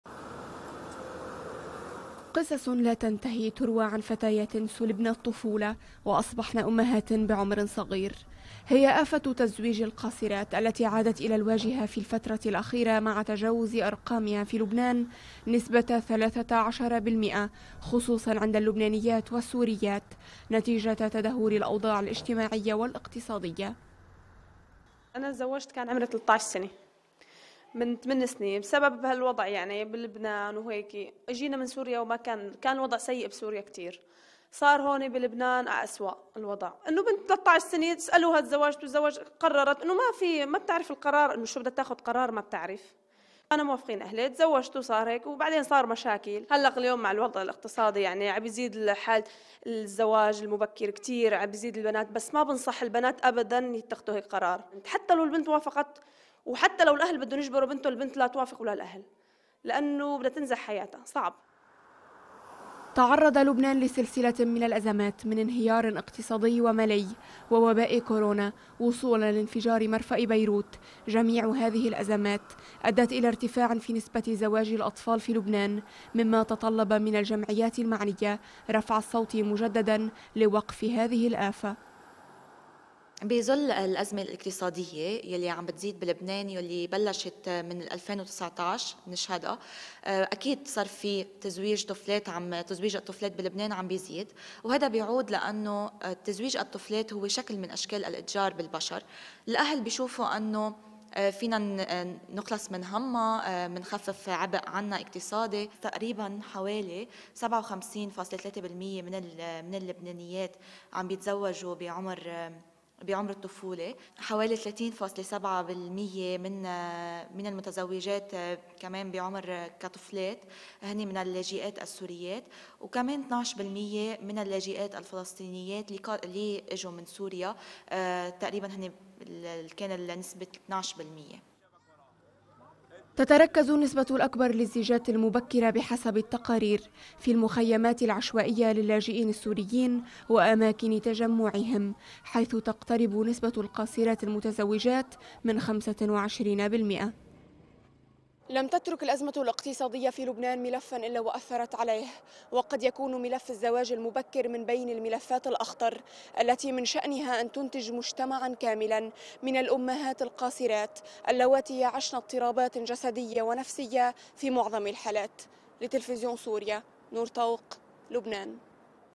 ما أسباب ارتفاع نسبة زواج القاصرات بين اللاجئات السوريات في لبنان؟
تشيرُ الأرقامُ بارتفاعِ نسبةِ زواجِ القاصرات لاسيما في مخيماتِ اللاجئين السوريين الذين تدهورت أحوالُهم أكثرَ خلال الفترةِ الأخيرة.. وبدأت عدةُ جمعياتٍ تناشدُ بوقفِ تزويجِ الفتياتِ اللواتي هنَّ تحت السنِّ القانون
تقرير: نور طوق